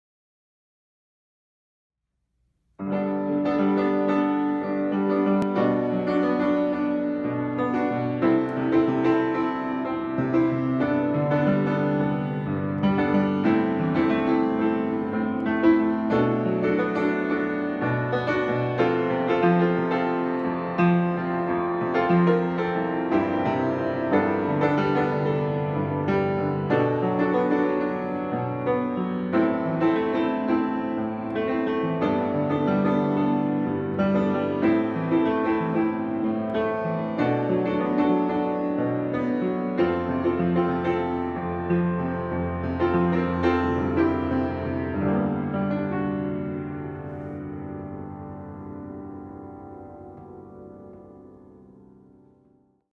Samick JS 131 Upright Piano
This Samick Upright Piano is in our showroom in El Dorado Hills, California, just East of Sacramento.

We buy & sell pianos, as well as offer pianos on consignment.